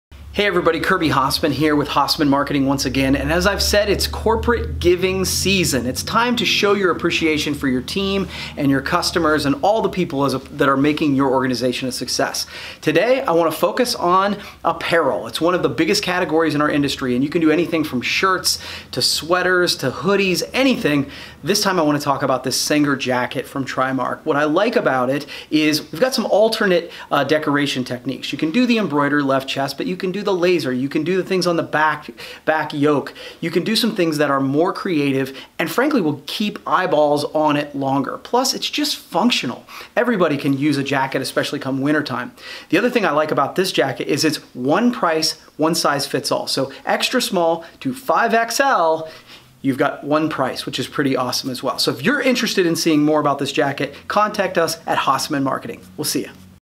Get THIS Senger Jacket for your team!
Want a great item for your team or your customers?  Then look no further than the jacket we did for our team at our Annual Trade Show.  The Senger jacket is light enough to be comfy but heavy enough to keep you warm...and has great decoration options!